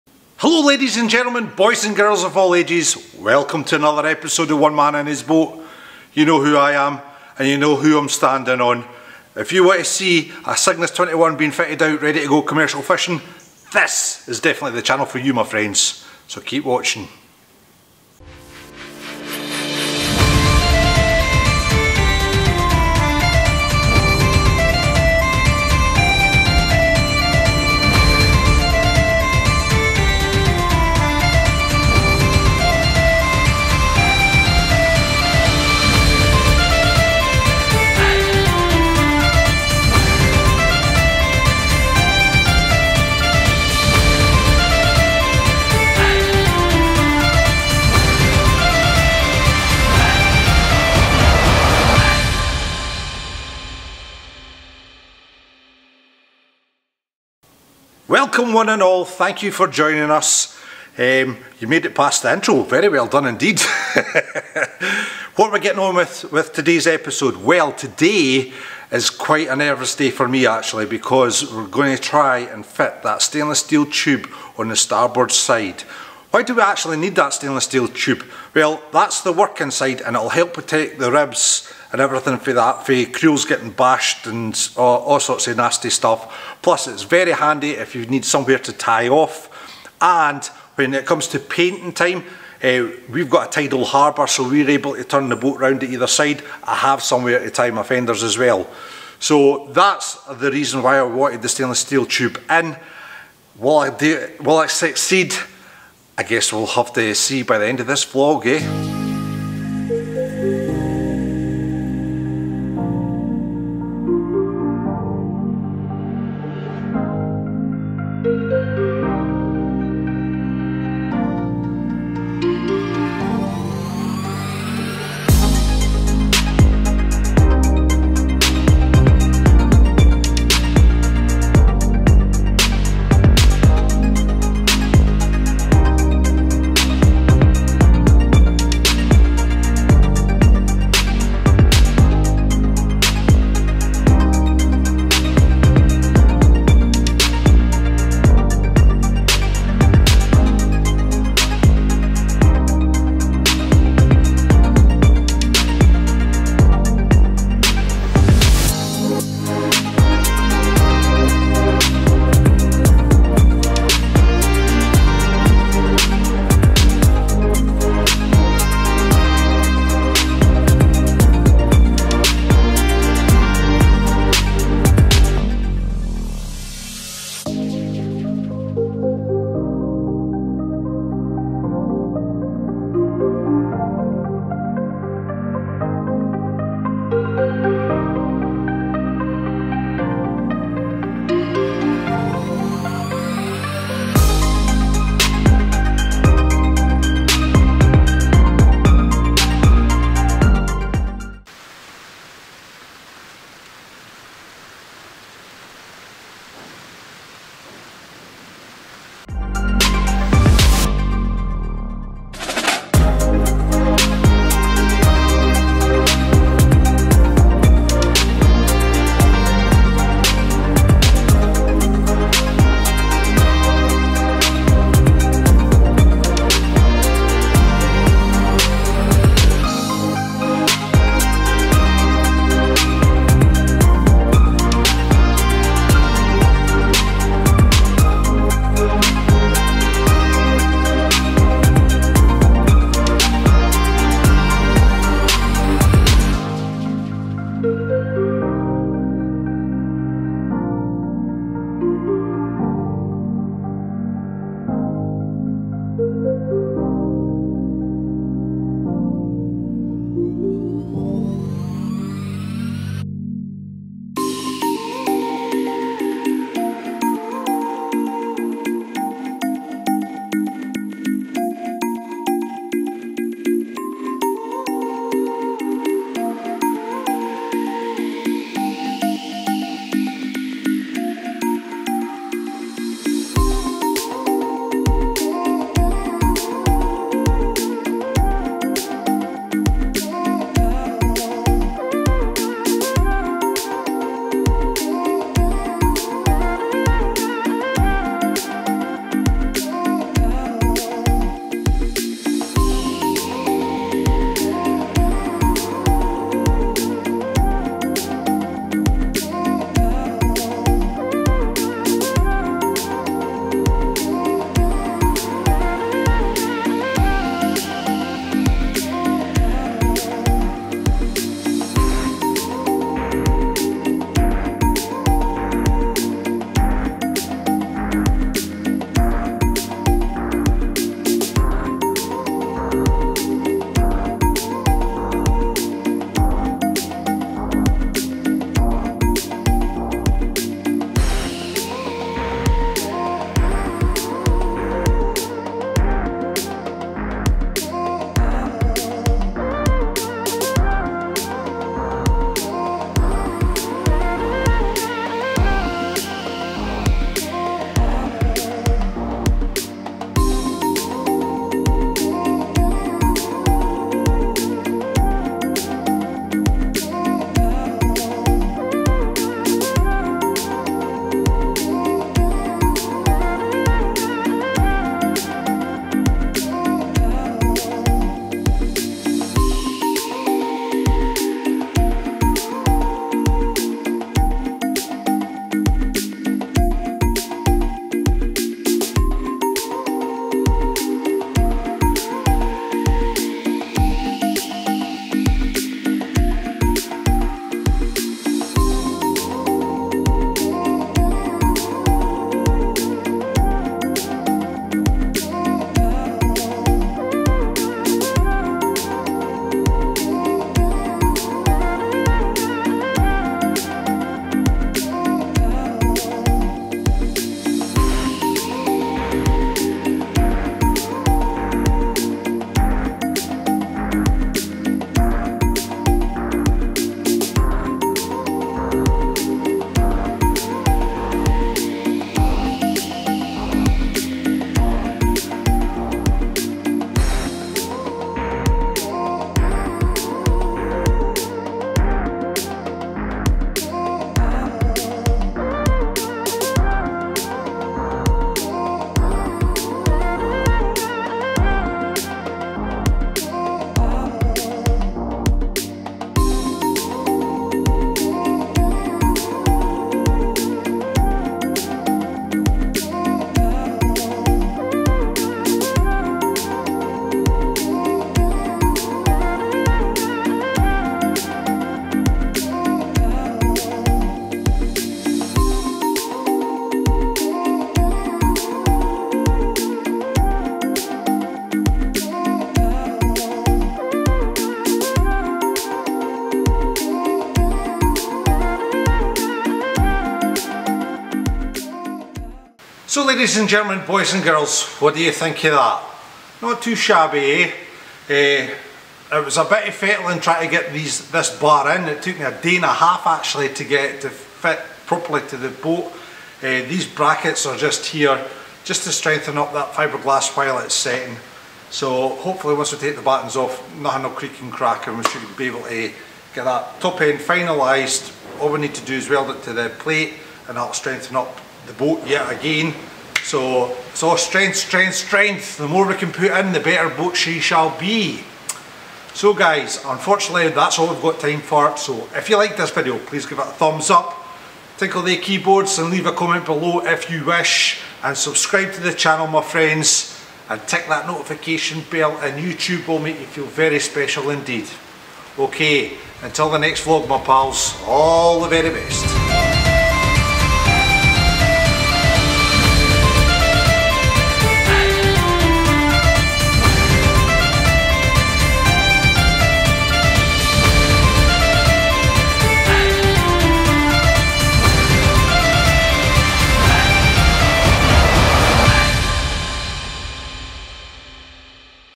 Tube fitting Episode 74
This week's vlog, we are fitting the Stainless steel rail, between the bottom of the gunnel, to the top of the deck.
Check out and see how i do this process, and what tool's jig's etc i use.